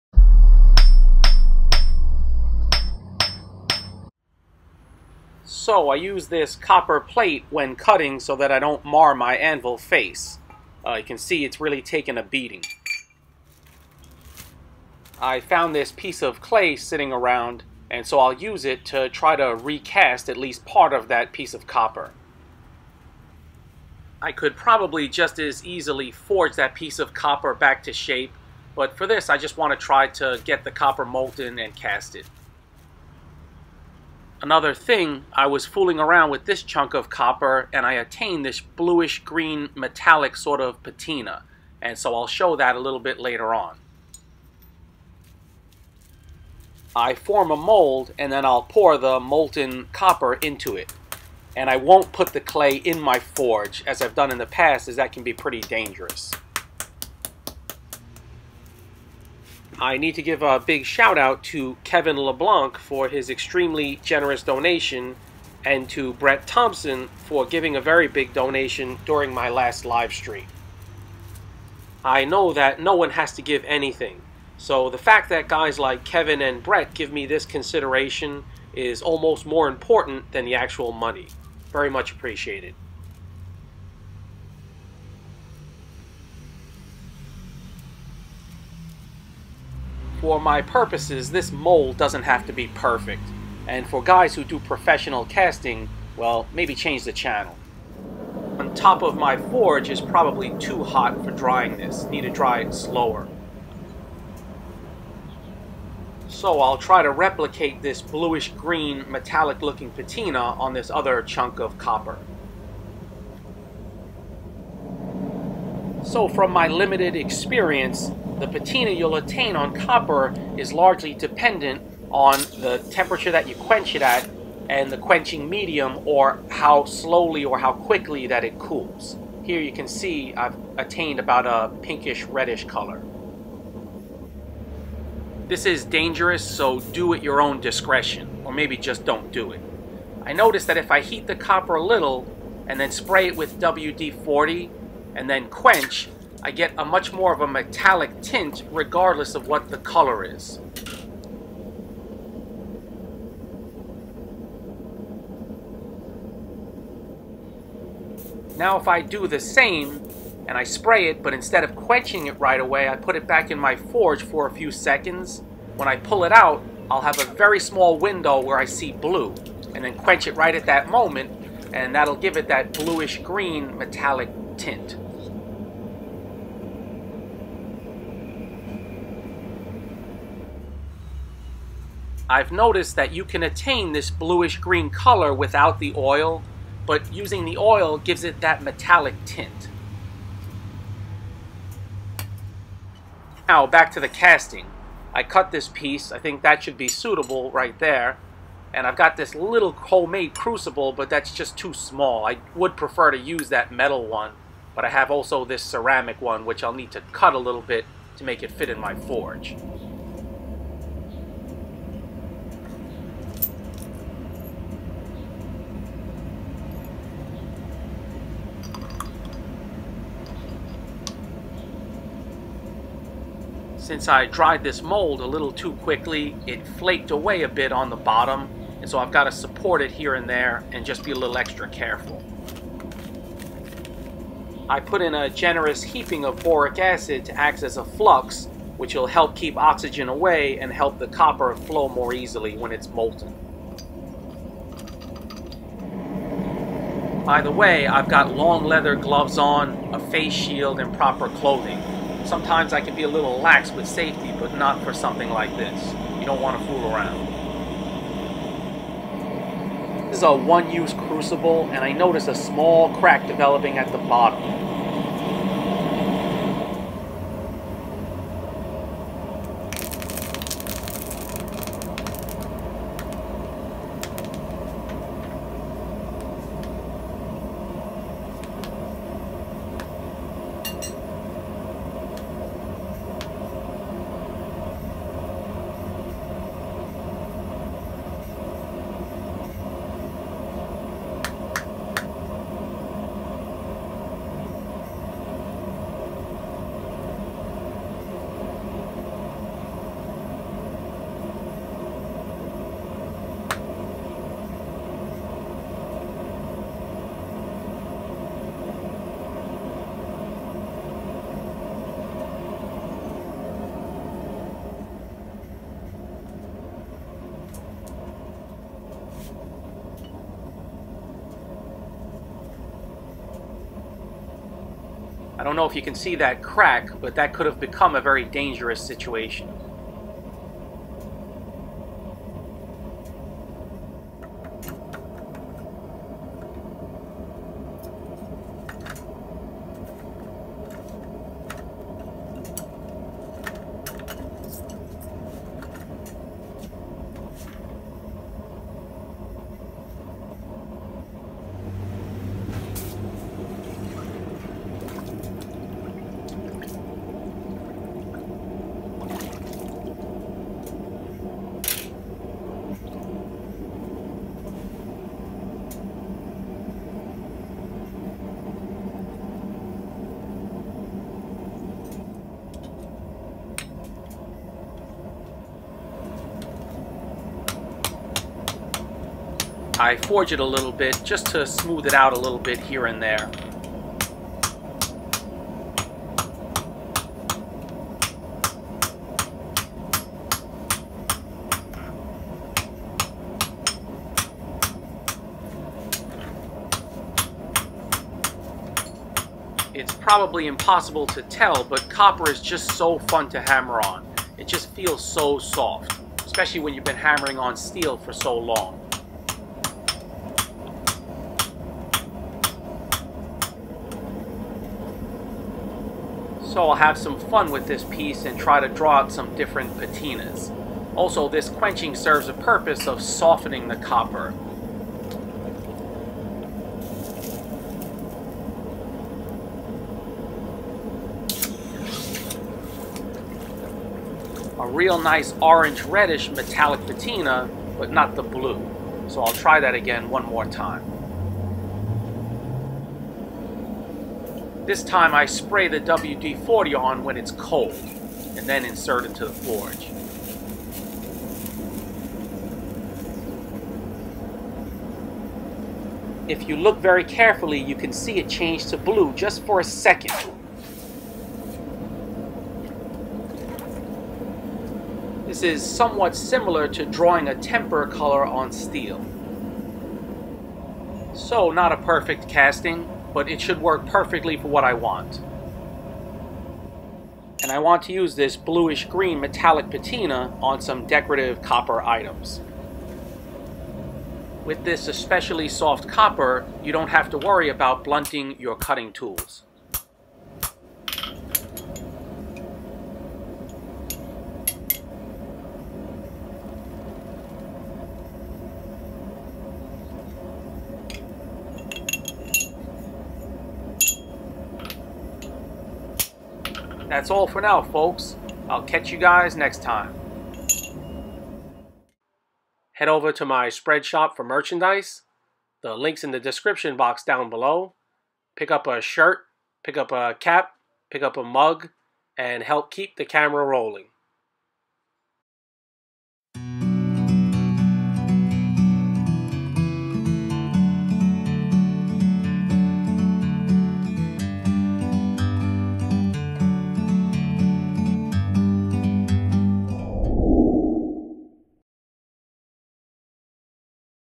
Copper Casting / Blue Green Patina
I have some fun melting some beat up copper and trying a different copper patina.

Thanks. Glen